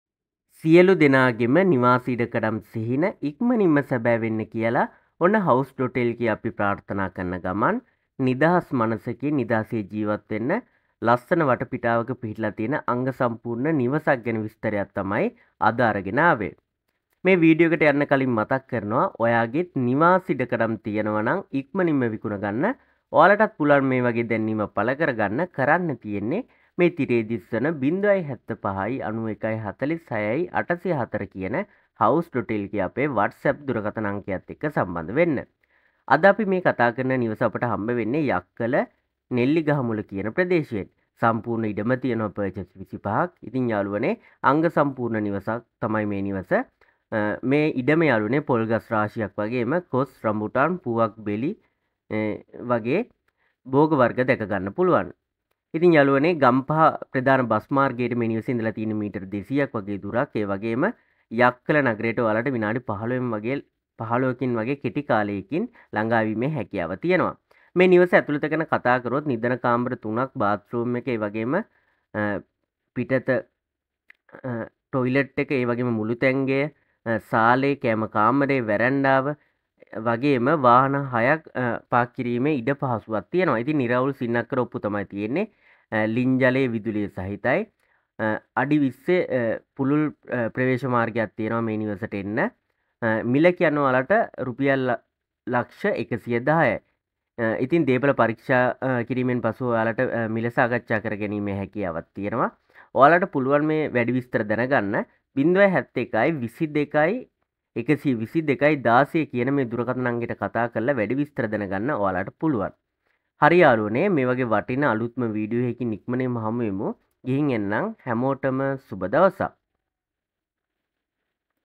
ලස්සන නිවසක් ගමු | Tour of Stunning House for Sale in Gampaha Yakkala |house.lk
Hey Friends👉

**Charming Fully Equipped House for Sale in Yakkala Nelligahamula**

Discover the perfect blend of comfort and natural beauty with this fully equipped house in the serene Yakkala Nelligahamula area. Set on a generous 25 perches of land, this property is surrounded by lush coconut trees and a variety of fruit trees including jackfruit, rambutan, and beli, offering a tranquil environment for a peaceful life.

**Property Highlights:**
- **Location**: Just 200 meters from the Gampaha bus road and only 15 minutes from Yakkala city, providing easy access to amenities and transportation.
- **Land Size**: 25 perches
- **Bedrooms**: 3 spacious rooms
- **Bathrooms**: 1 indoor, 1 outdoor
- **Kitchen**: Well-equipped for your cooking needs
- **Living Room**: Comfortable and inviting
- **Lust Room**: Additional space for relaxation
- **Veranda**: Perfect for enjoying the outdoors
- **Parking**: Ample space for up to 6 vehicles
- **Driveway**: 20 feet wide for easy access

Additional features include freehold deeds, well water, and electricity, ensuring all essential utilities are available. The property’s clear surroundings and beautiful setting create the ideal environment for a free and relaxed lifestyle.

**Price**: 110 Lakhs (negotiable after inspection)

ඔයාගෙත් නිවාස ඉඩ කඩම් ඉක්මණින් විකුණ ගන්න නොමිලේ දැන්වීමක් පල කර ගන්න දැන්ම අපට කතා කරන්න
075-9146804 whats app